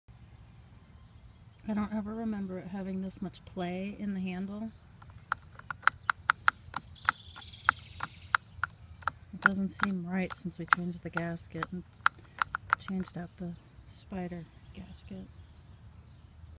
Loose Multiport
Trying to figure out a gradual pressure loss